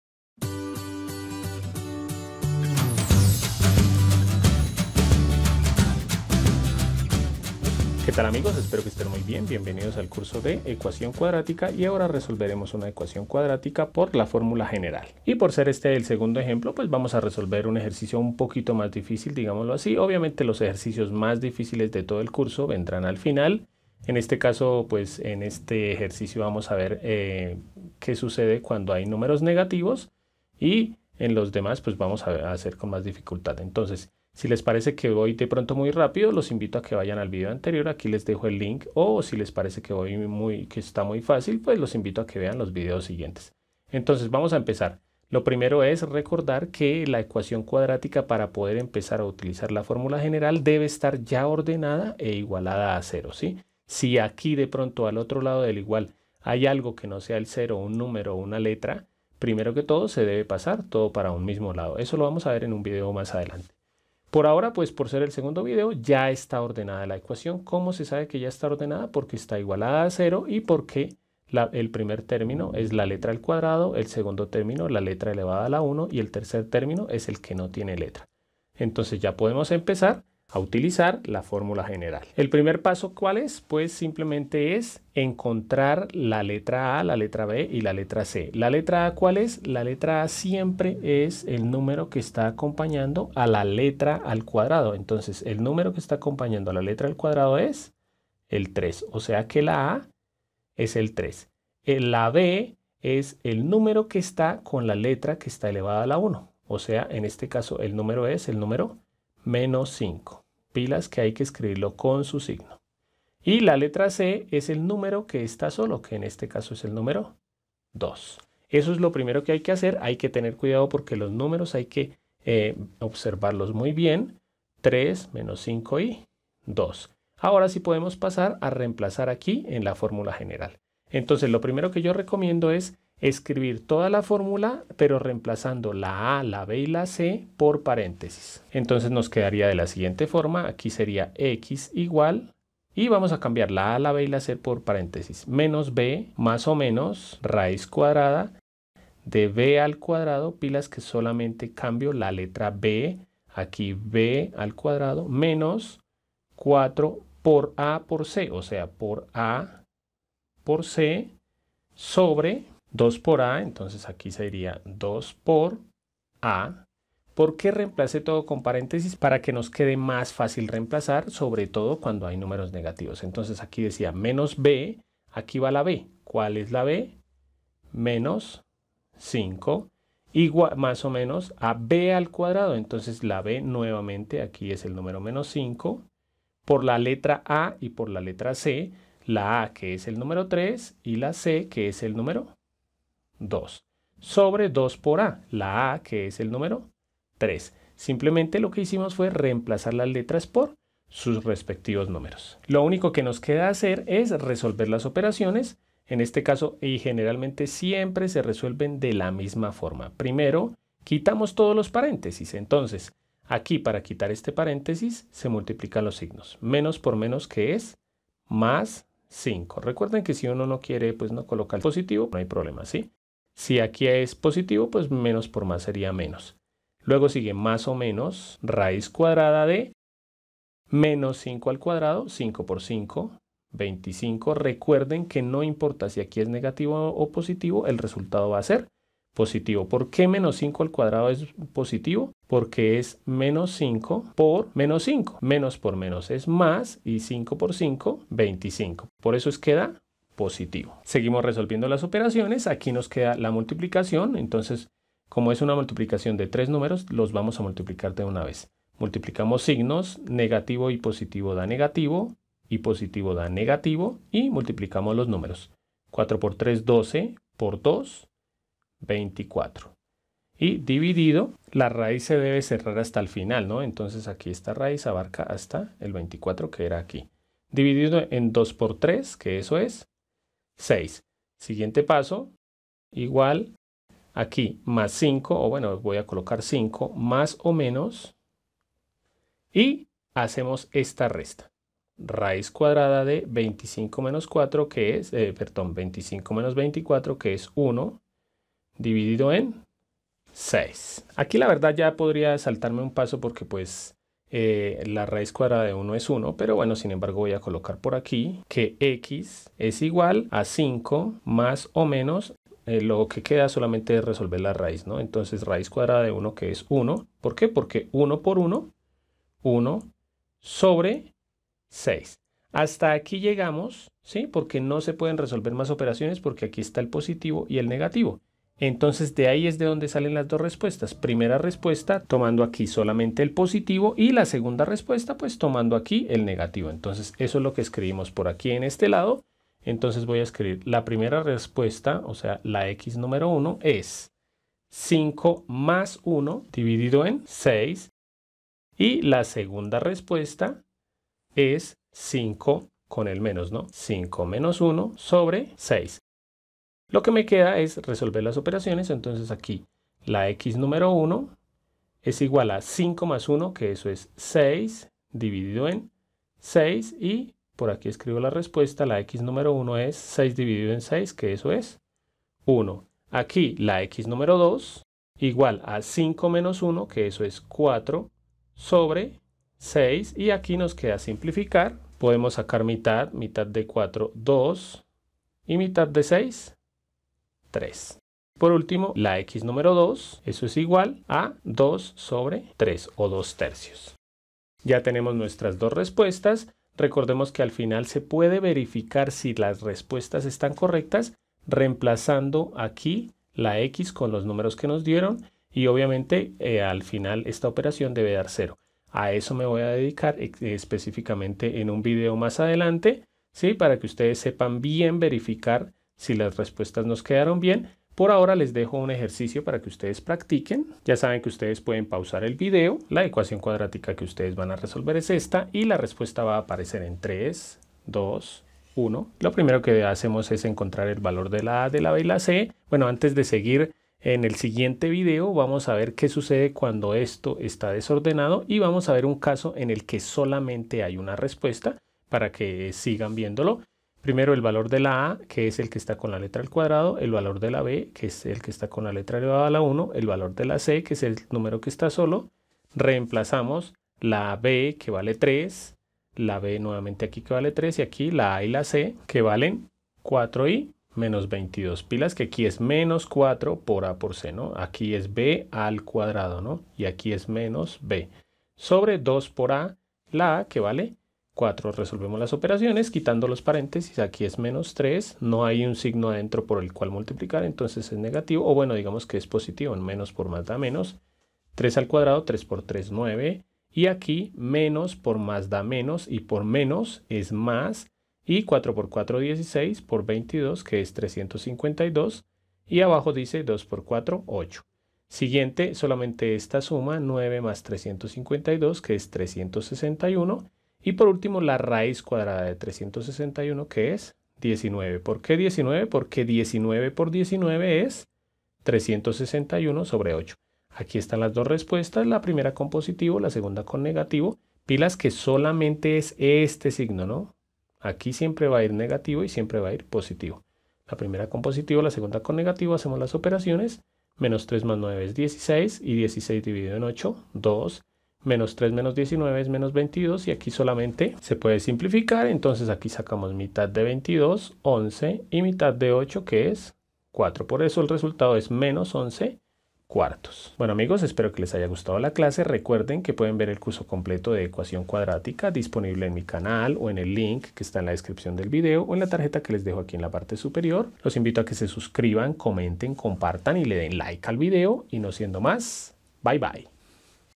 Ecuación cuadrática por fórmula general | Ejemplo 2
Explicación detallada de la forma de resolver una ecuación cuadrática, segundo ejemplo de la solución por la formula general, dentro del curso de ecuación cuadrática.

0:00 Saludo
0:15 Conceptos que debes saber
1:20 Solución del ejemplo
7:05 Ejercicio de práctica